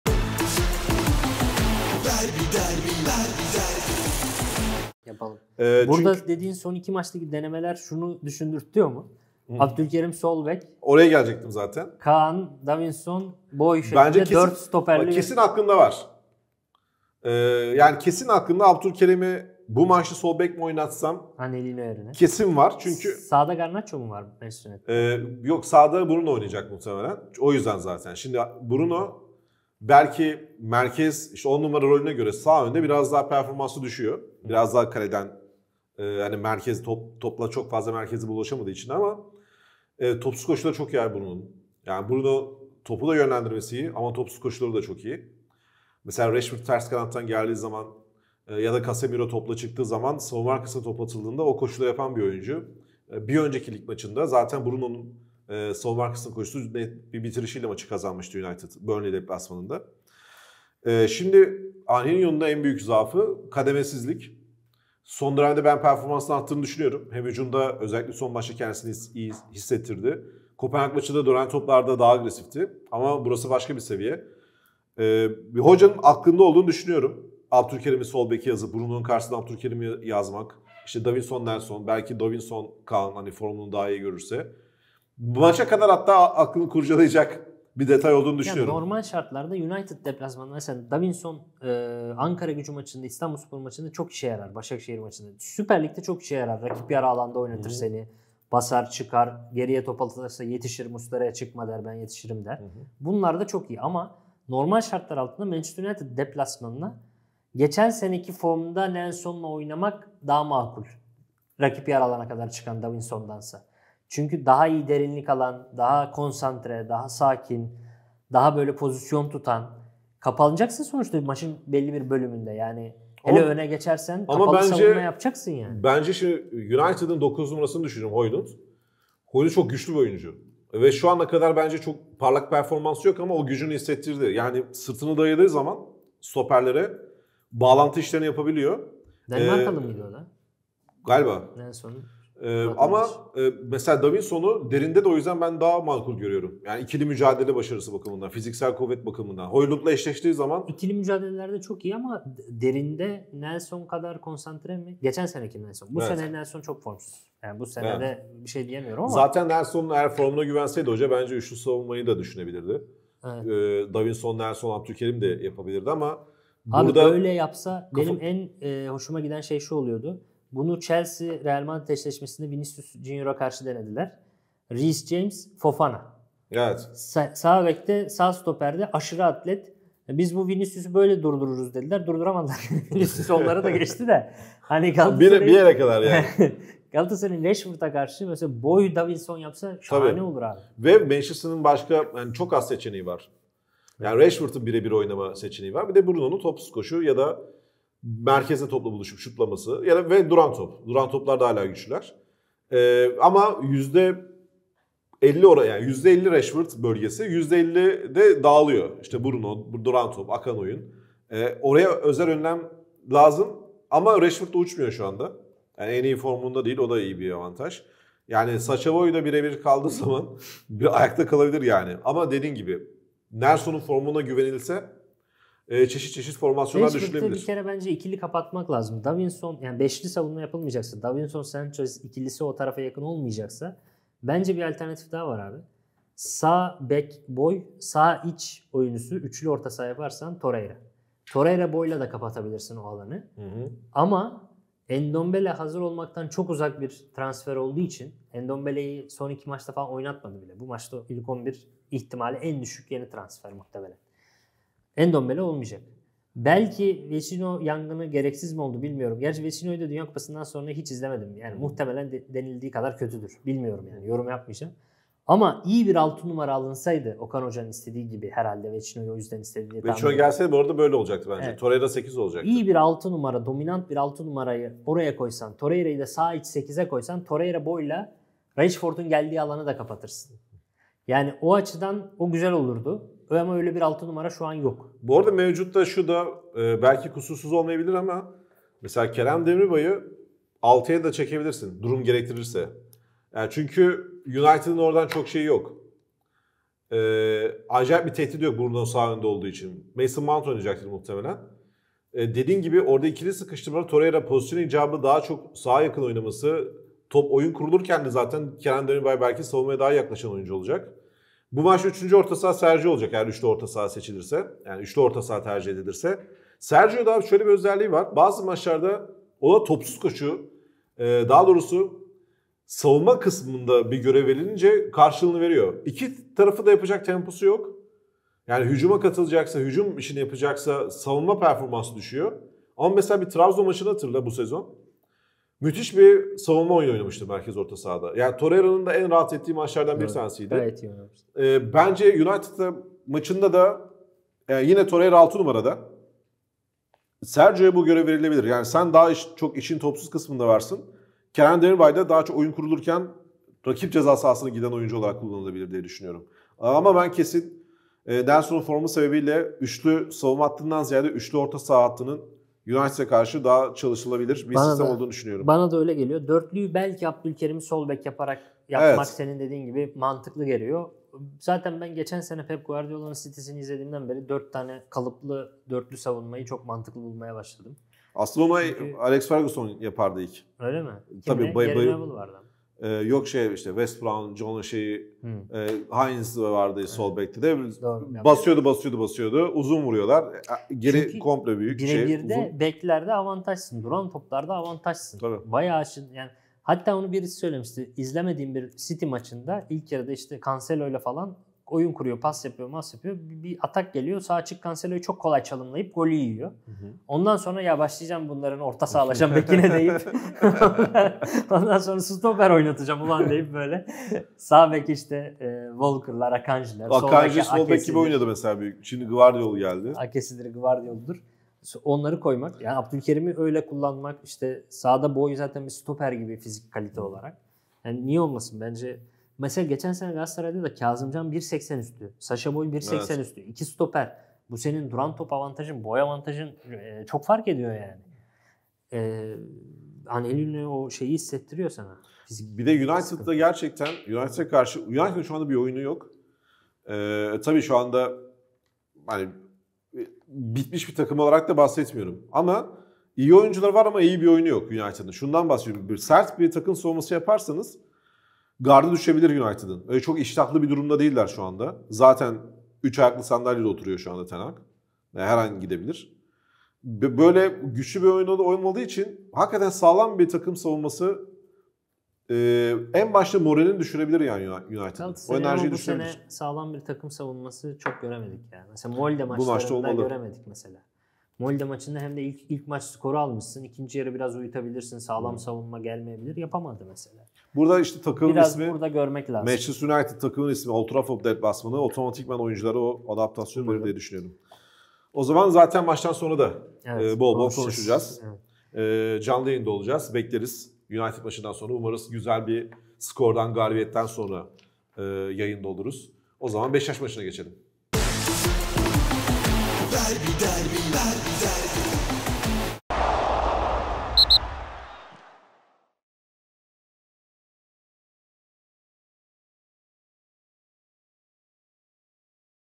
Galatasaray, Manchester United’a karşı sağlam bir takım savunması yapmalı | Mustafa | İlk Raund #4
Mustafa Demirtaş ve Sinan Yılmaz, Derby'nin sunduğu İLK RAUND’da Galatasaray'ın Manchester United maç öncesini değerlendirdi.